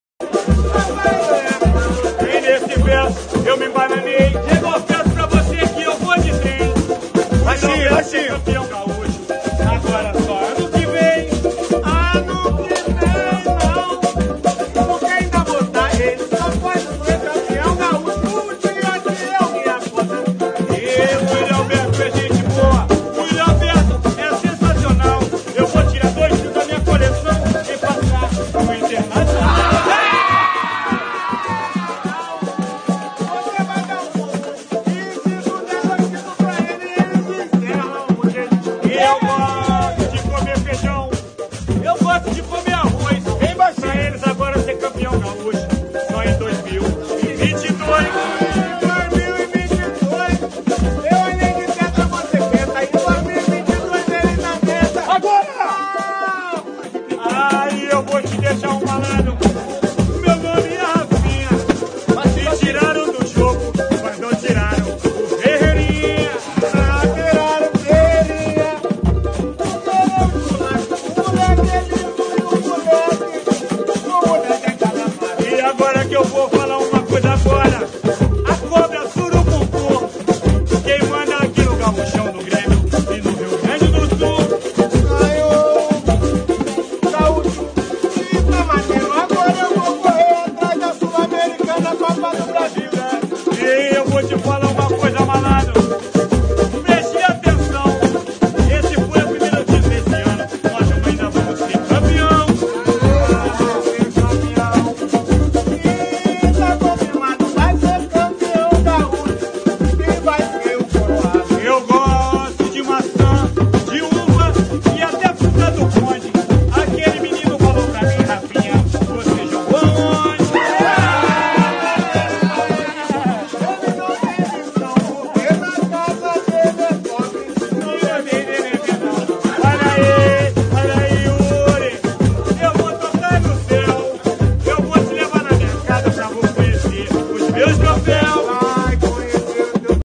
RAFINHA e DIEGO SOUZA CRIAM MÚSICA E ALFINETAM INTER! PAGODE DOS GURI DO GRÊMIO! SENSACIONAL hahaha!
QUE RESENHA HAHAHA RAFINHA E DIEGO SOUZA TIRARAM ONDA FAZENDO UM PAGODE!